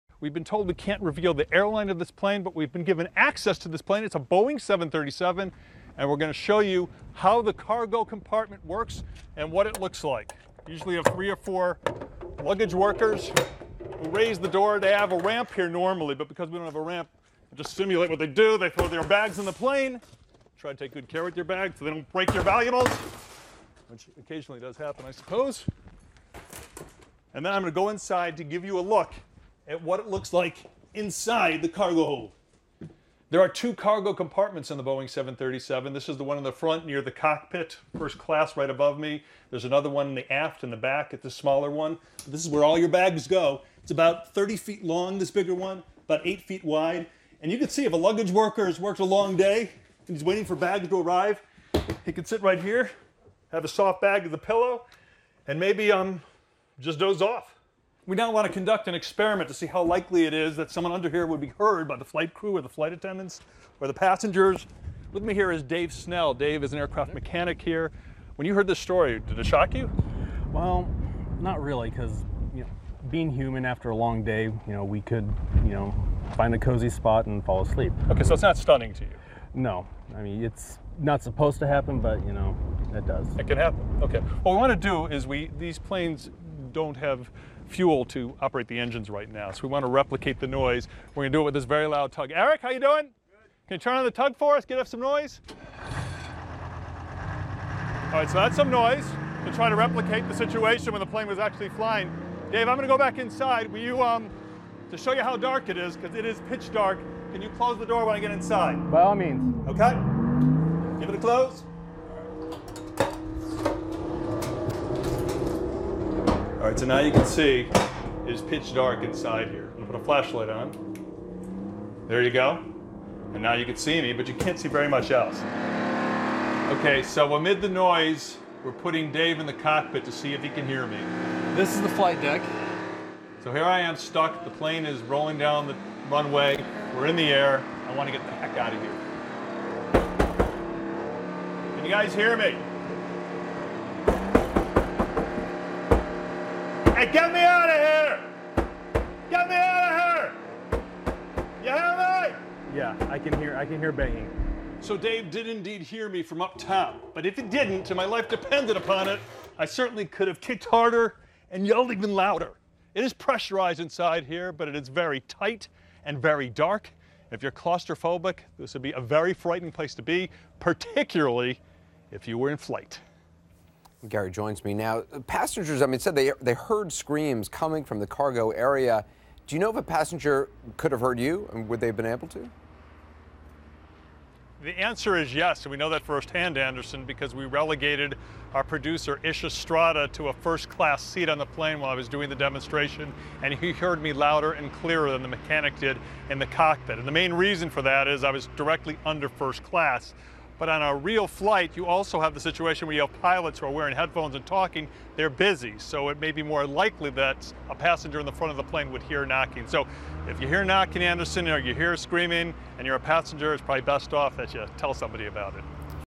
What's it like inside a plane's cargo hold?
CNN's Gary Tuchman goes inside a plane's cargo hold to see what it feels like to be trapped inside and survive a flight.